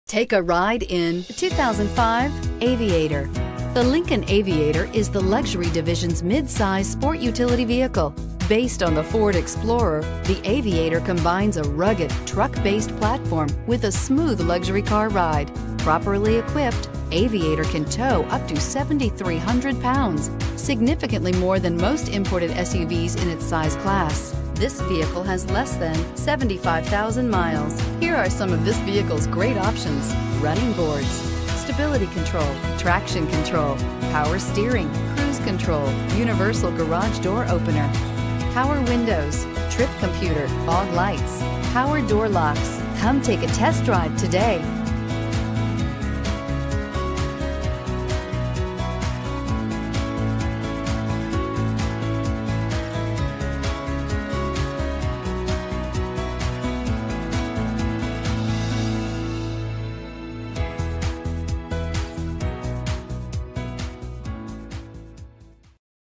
2005 Lincoln Aviator Houston TX 8P005A2
2005 Lincoln Aviator Luxury

Recent Arrival! Clean CARFAX. Red 2005 Lincoln 4D Sport Utility Aviator 13/18mpg RWD 4.6L V8 DOHC 5-Speed Automatic with Overdrive Luxury